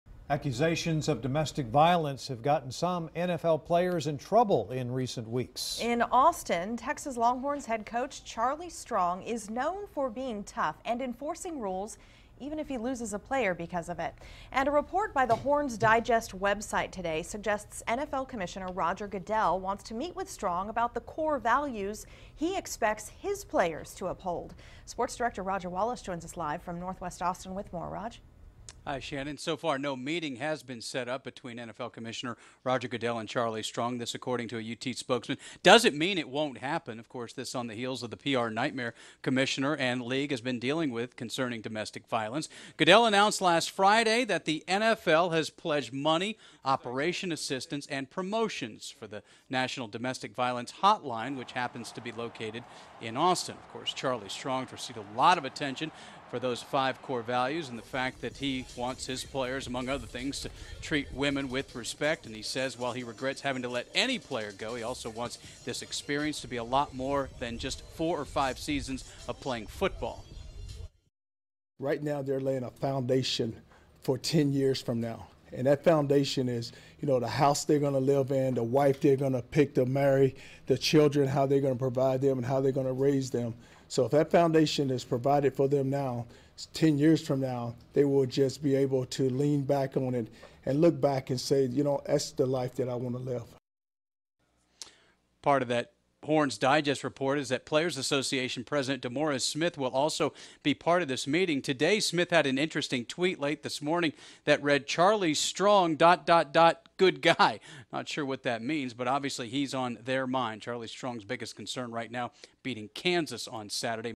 NFL leaders to meet with UT coach Charlie Strong, sources say
NFL leaders are reportedly set to meet with University of Texas Longhorns Head Football Coach Charlie Strong during a trip to Austin.